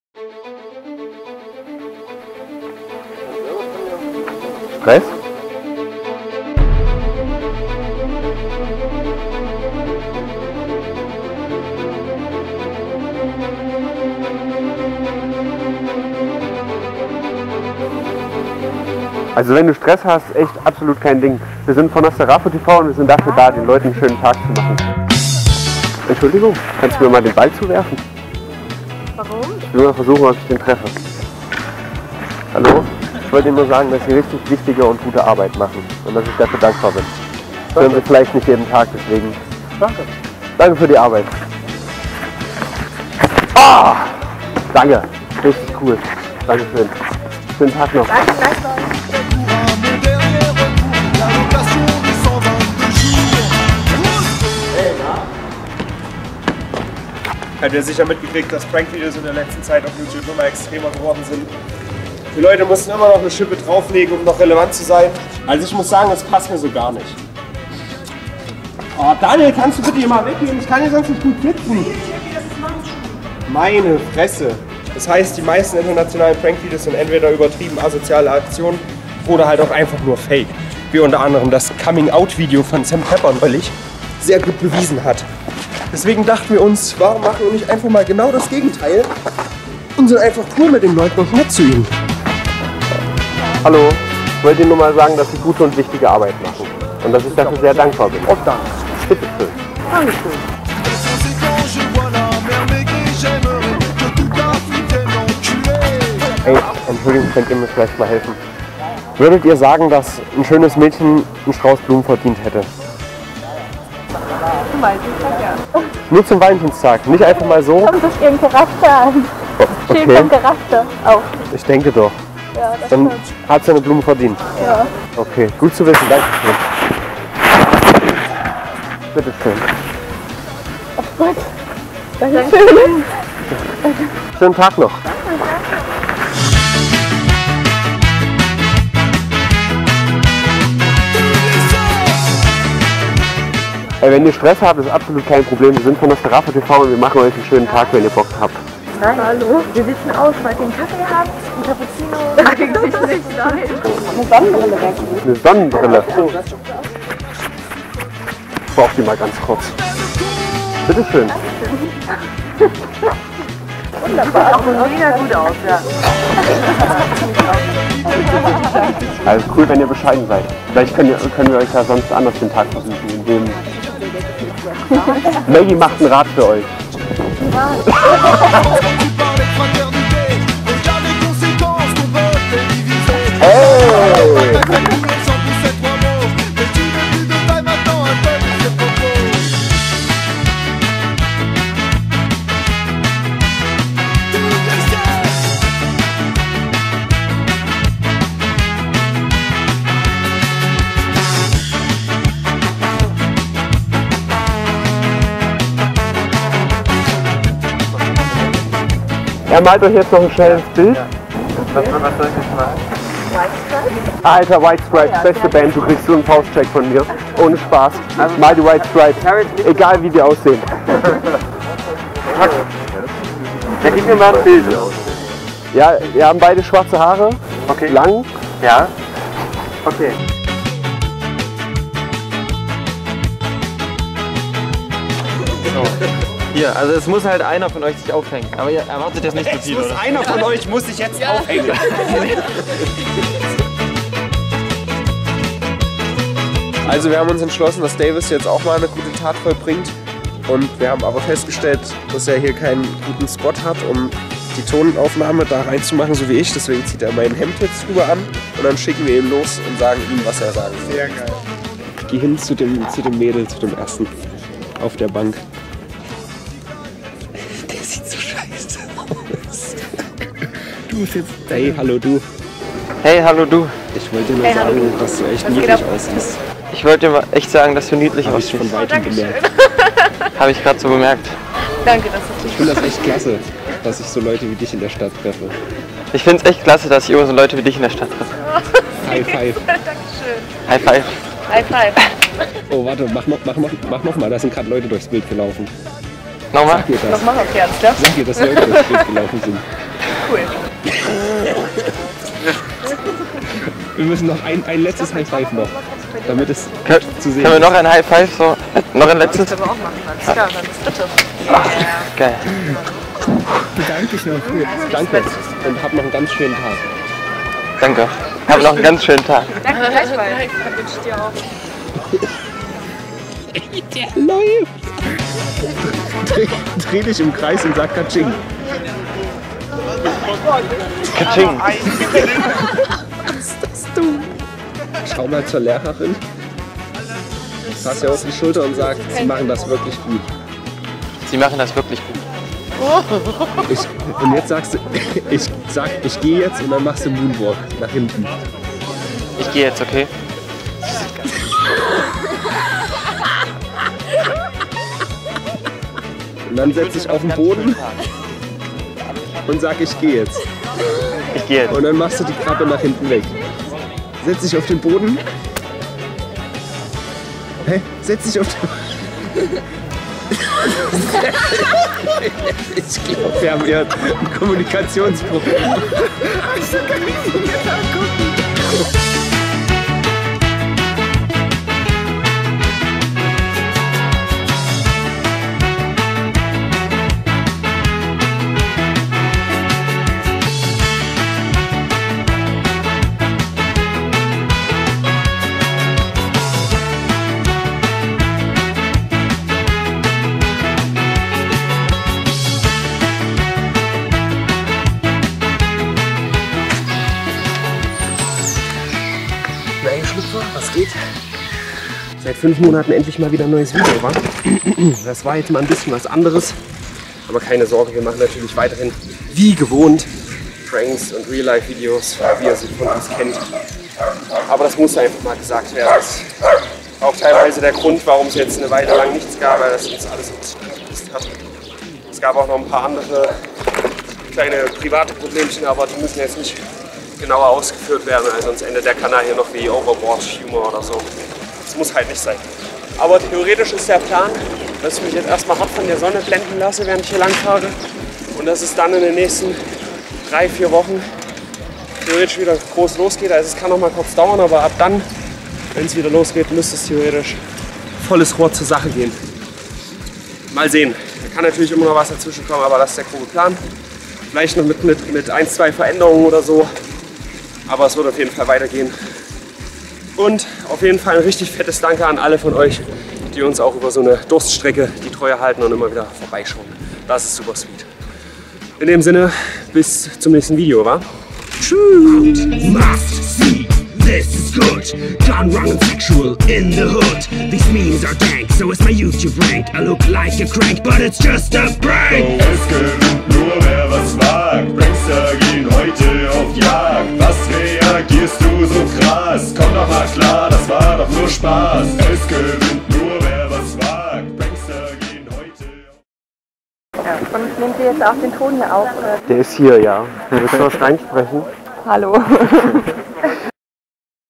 Making Pranks Great Again (NosTeraFuTV)
Over the last couple of years pranks became more and more extreme- this is how we keep up with that.
And no, this video has nothing to do with Trump...
Neither being a complete asshole to strangers, nor faking anything is something we ever did and want to do.
More videos coming very soon.